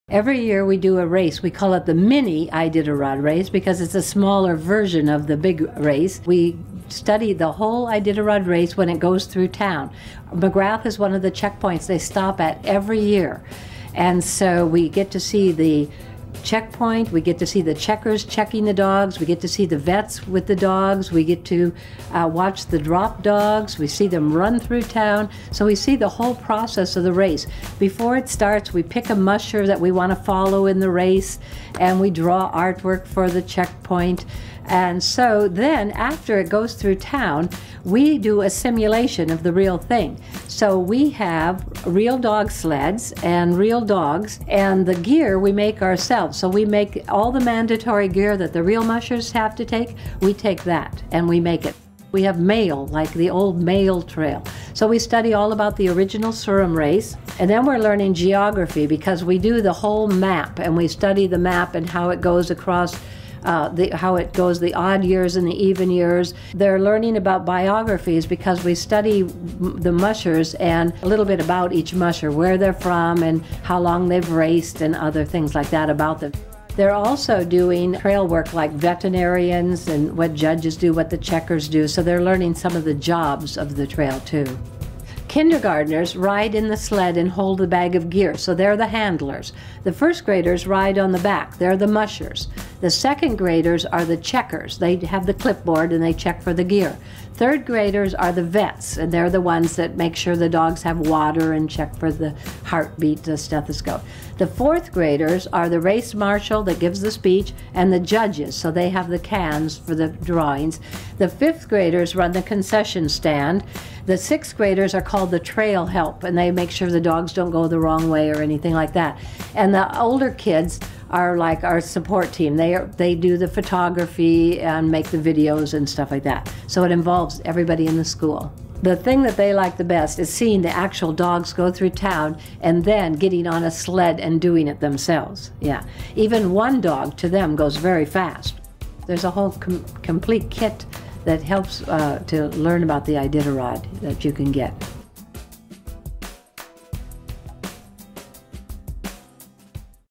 Iditarod in Education: McGrath's Mini Iditarod
Principal Judy Kuhn at the McGrath school uses the Iditarod to teach math, history, and social studies. Hear how see does it.